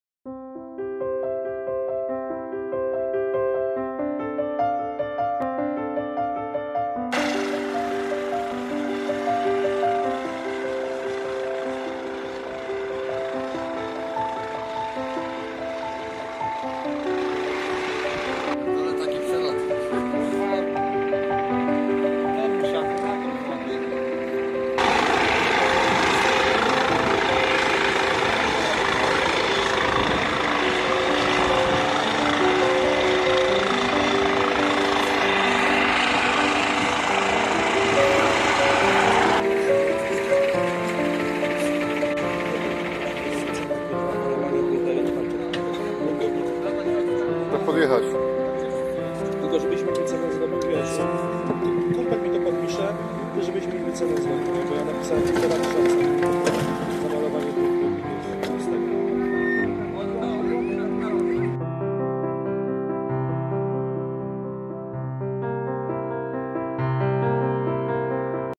Wypadek Drogowy Pomoc Drogowa Dąbrowa Górnicza Laweta Gołonóg
Wypadek drogowy na ul.Aleja Józefa Marszałka Piłsudskiego Dąbrowa Górnicza

Pomoc Drogowa Auto Laweta Hol Holowanie Samochodów
Fast auto hol szybki mega serwis